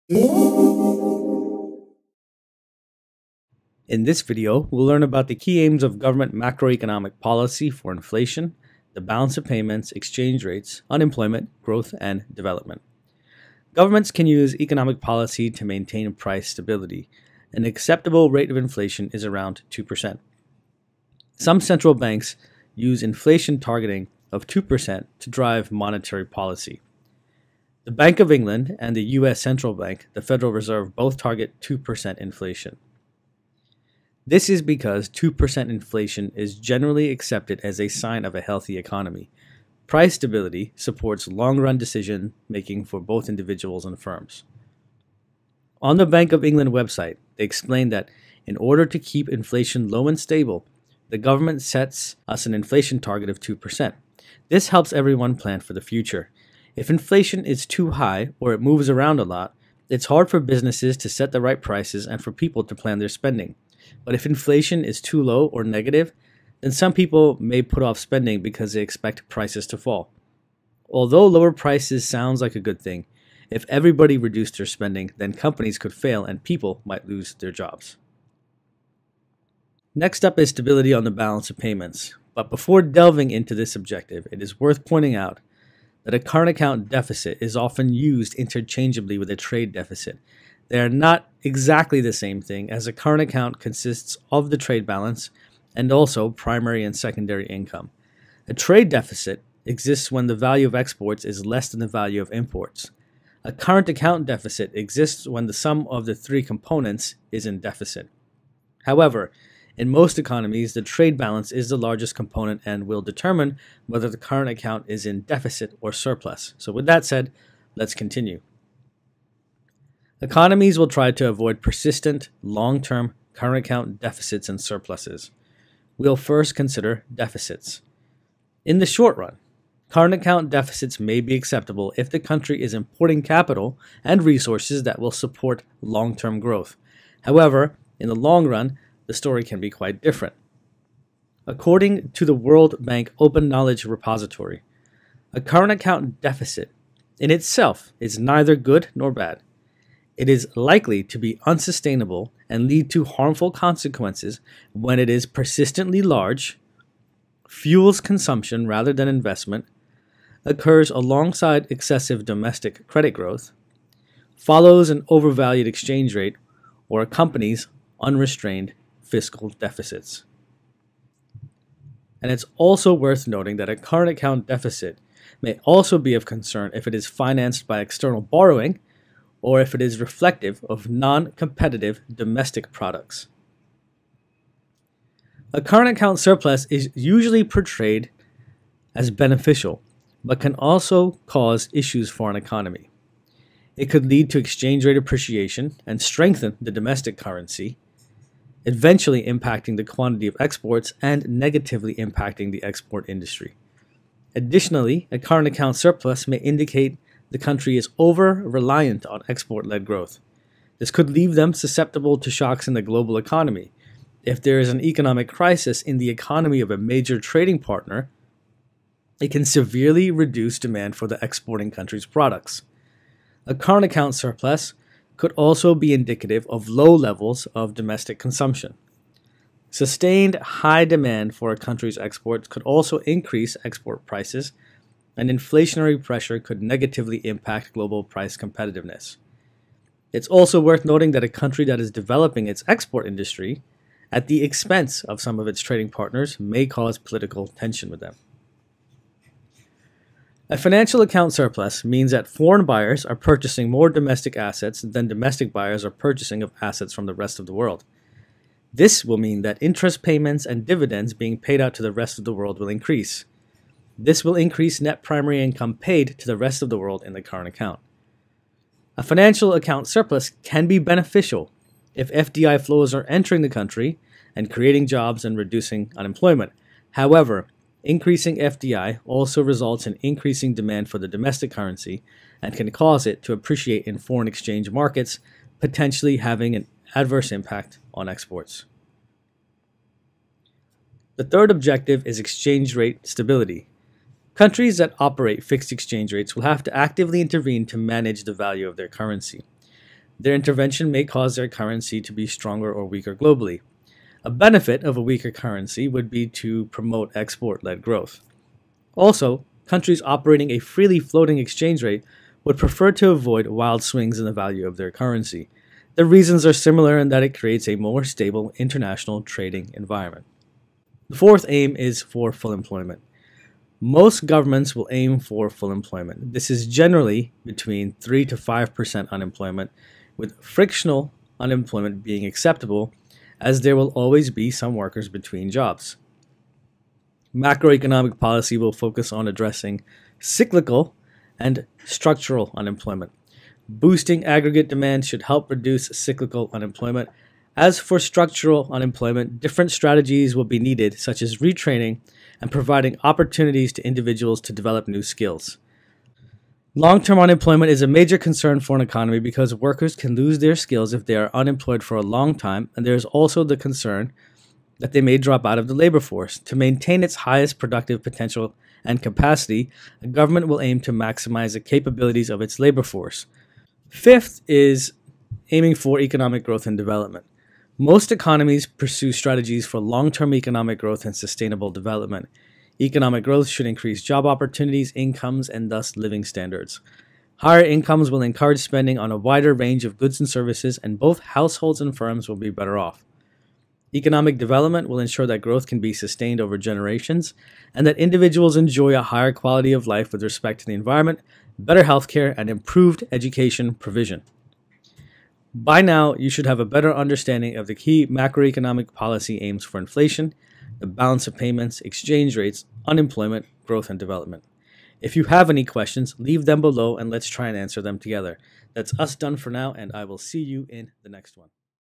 Government macro policy aims
In this video, you'll learn about the key macro policy aims of government.

Access 's free comprehensive notes on government macro policy aims